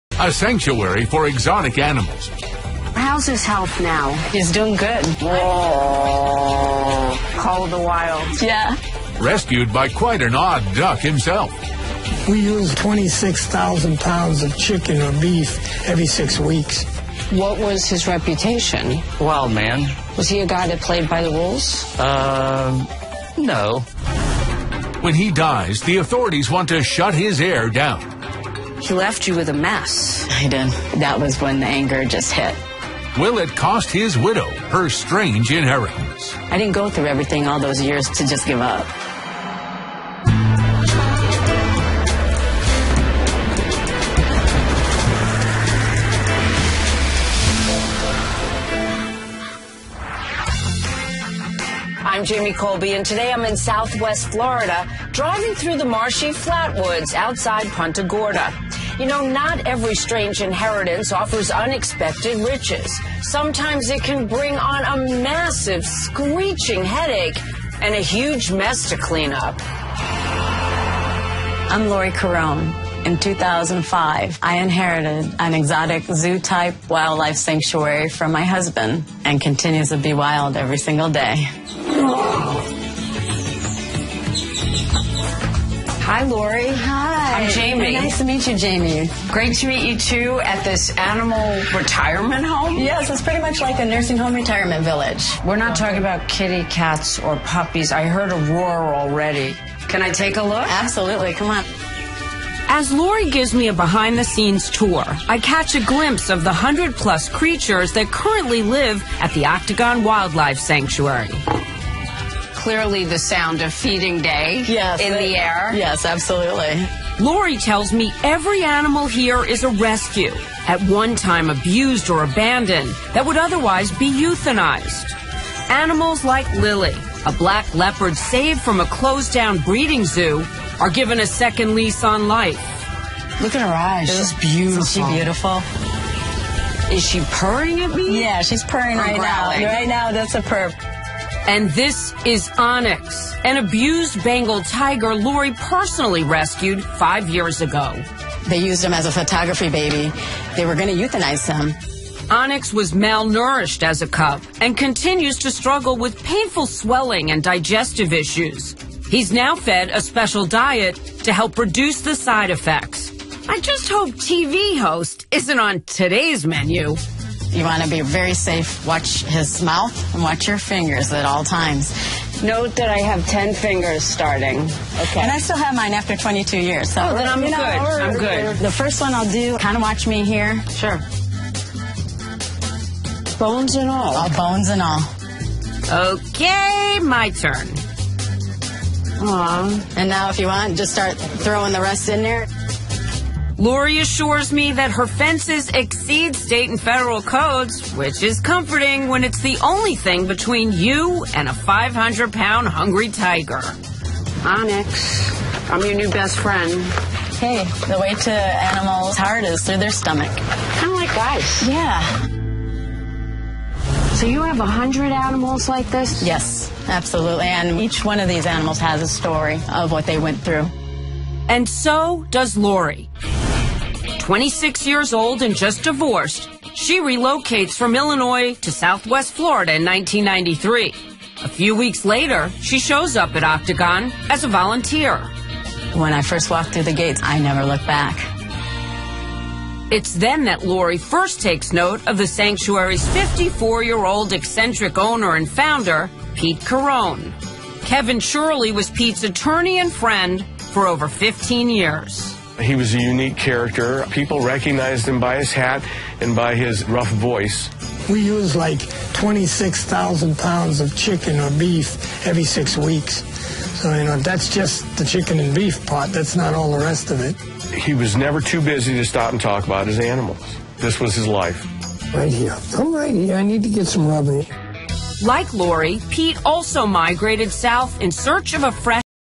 Saving exotic animals at a Florida wildlife refuge
Host Jamie Colby takes a trip to a Southwest Florida wildlife refuge of exotic animals.